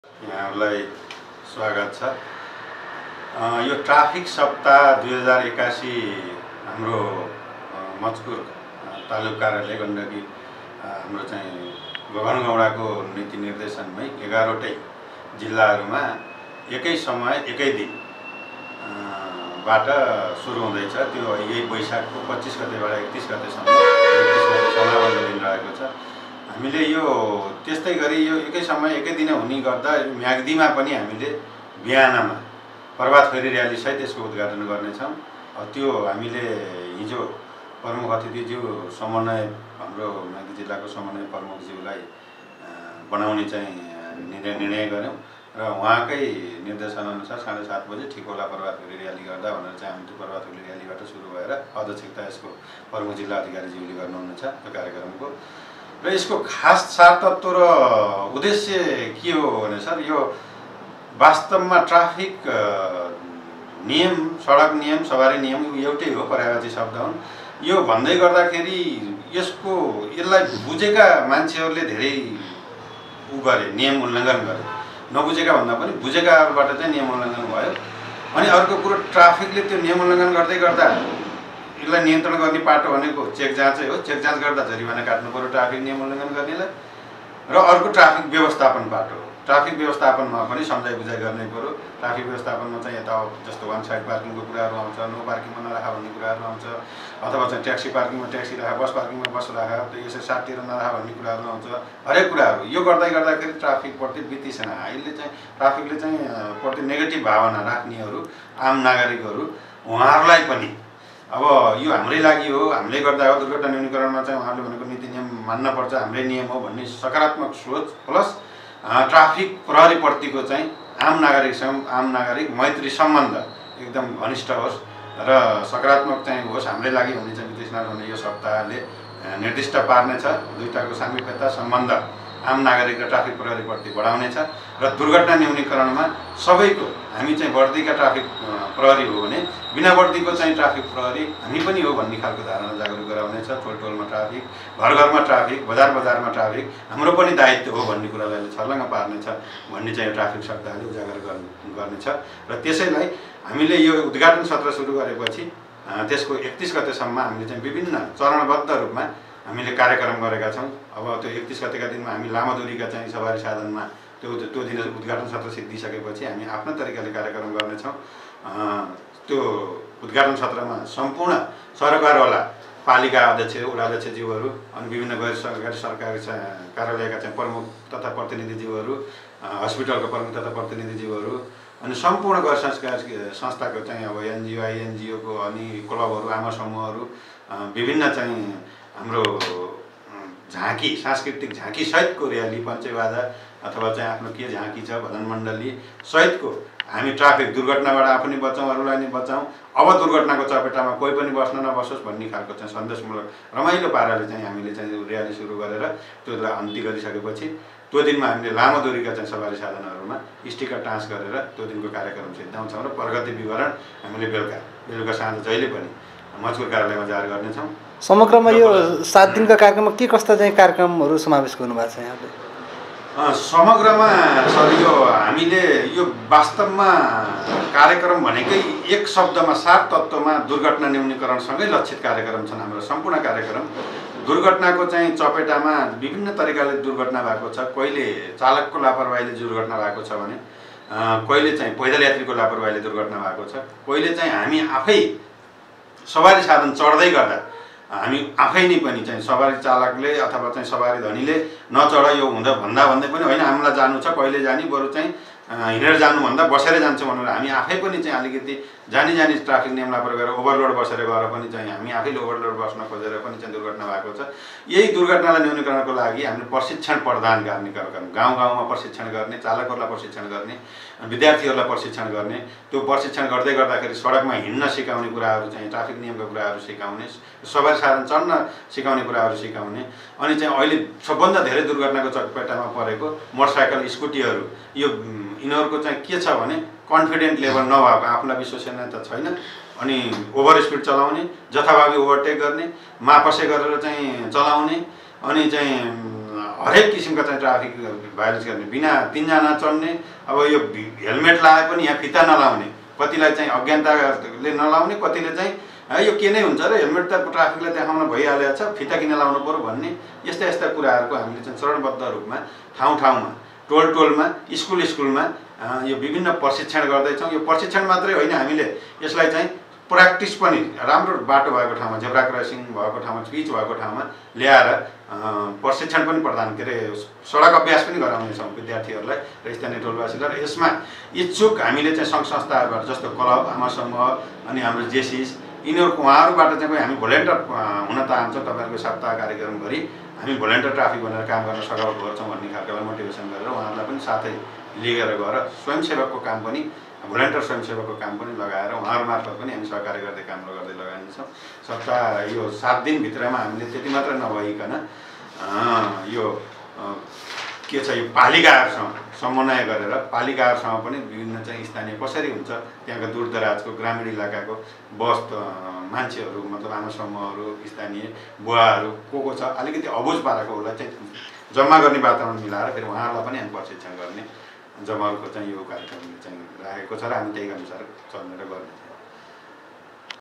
म्याग्दीमा सचेतनामुलक कार्यक्रम गरेर ट्राफिक सप्ताह–२०८१ मनाइने । Raj kumar kc
म्याग्दीमा सचेतनामुलक कार्यक्रम गरेर ट्राफिक सप्ताह–२०८१ मनाइने भएको छ । दुर्घटना न्यूनिकरण र ट्राफिक नियमको पालनालाई प्रभाबकारी बनाउन वैशाख २५ देखि ३१ गतेसम्म विभिन्न सचेतनामूलक कार्यक्रम आयोजना गरेर मनाउन लागिएको ट्राफिक सप्ताहको बारेमा जानकारी गराउँदै जिल्ला ट्राफिक प्रहरी कार्यालयका प्रमुख, प्रहरी बरिष्ठ नायब निरिक्षक राजकुमार केसी